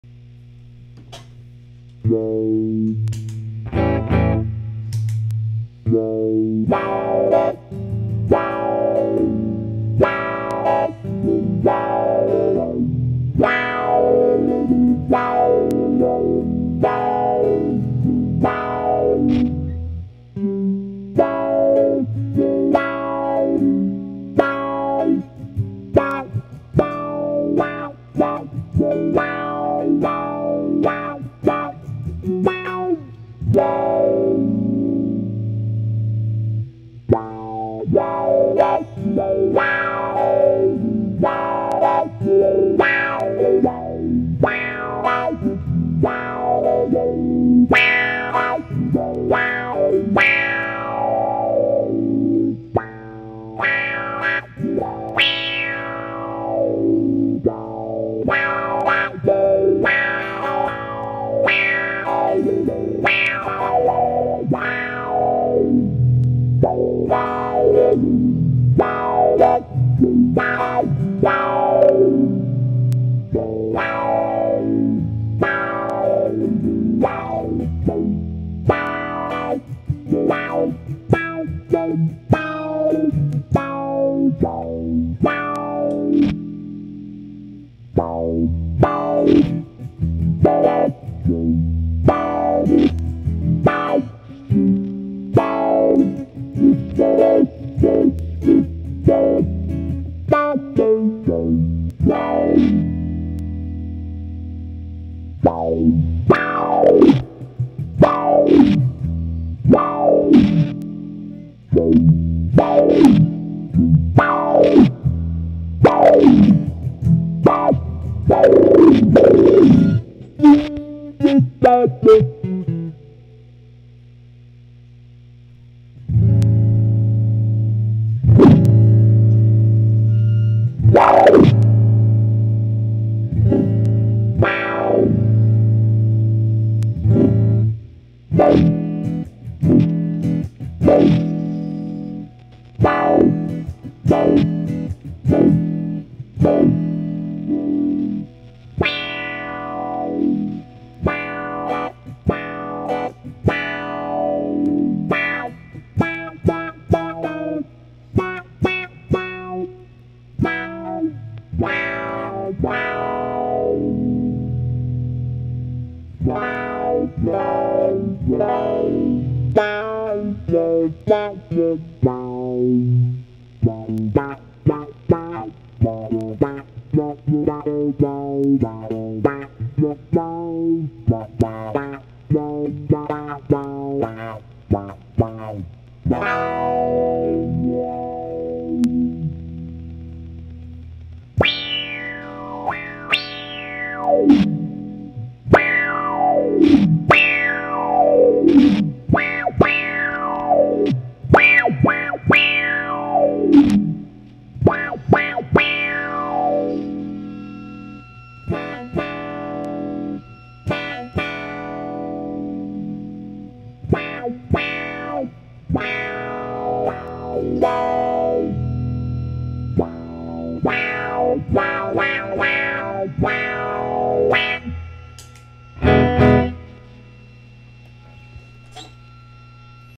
Envelope Filter
Based on the Maestro Envelope Filter.
Controls:
Resonance
Attack
Decay
Filter